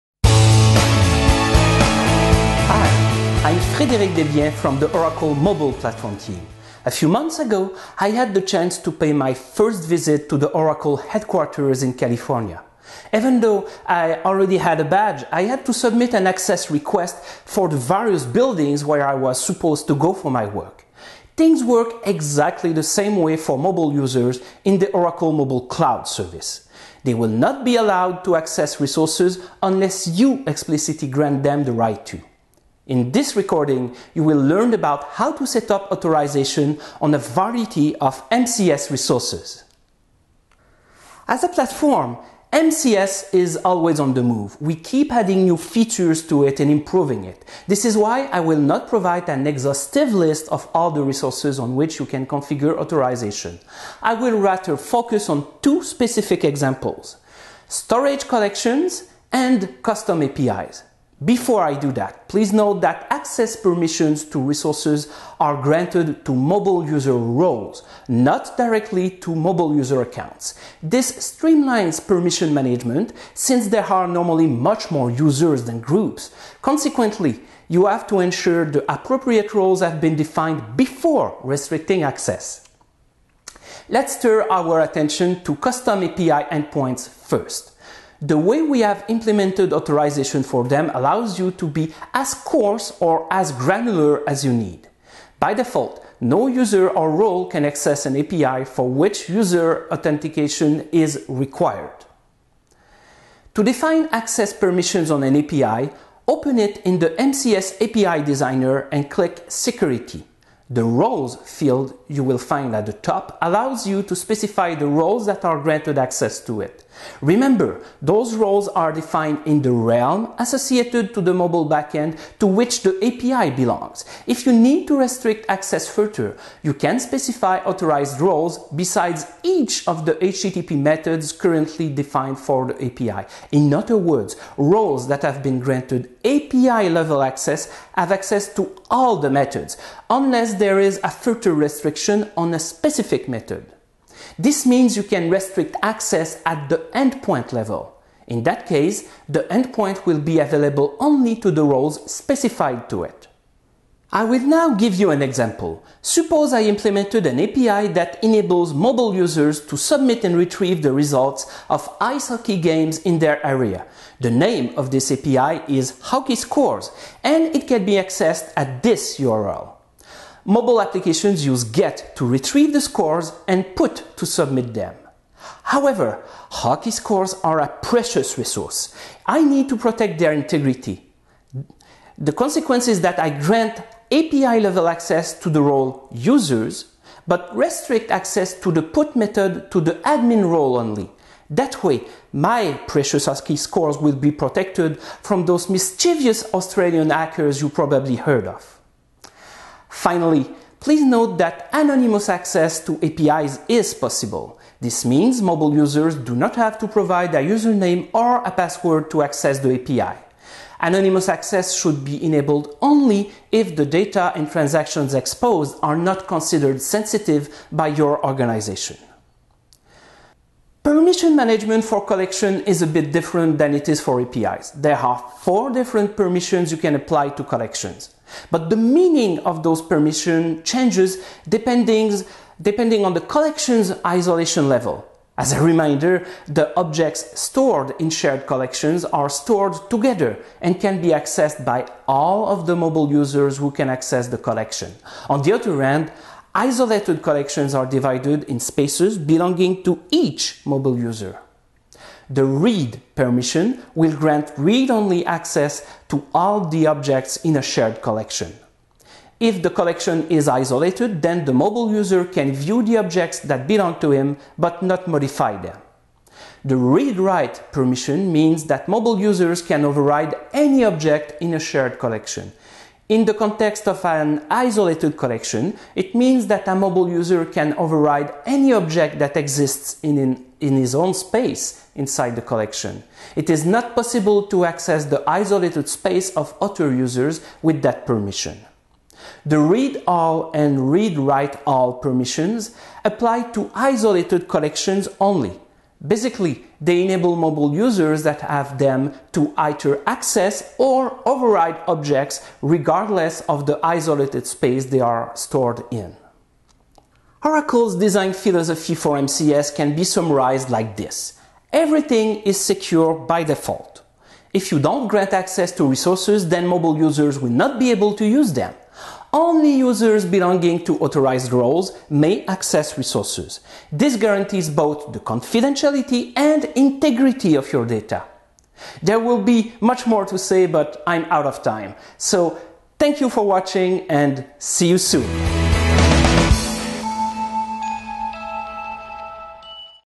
MCS: 18. Restricting Access to MCS Resources
In the Oracle Mobile Cloud Service, resources are secure by default. Nobody can access them unless you authorize access to at least one user role. This episode will teach you how to setup authorization on mobile storage collections as well as custom API endpoints.

Like to learn more about Oracle Mobile Cloud Service?